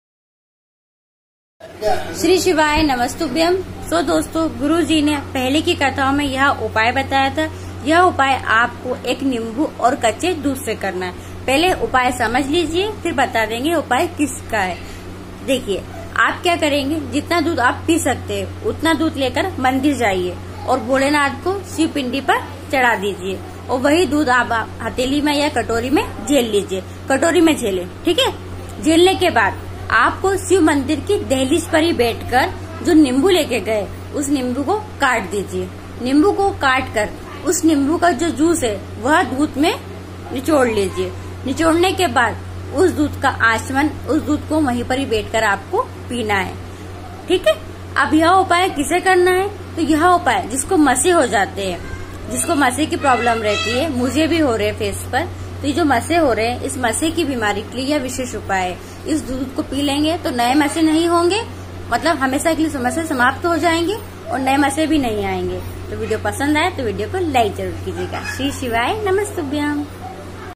केवल नींबू से करे यह उपाय - कोई भी कर सकता है
Thanks
नोट :- गुरुजी पंडित प्रदीप मिश्राजी के दवारा शिव महापुराण कथा में बताए गए उपाय हमने समझाने की कोशिश की है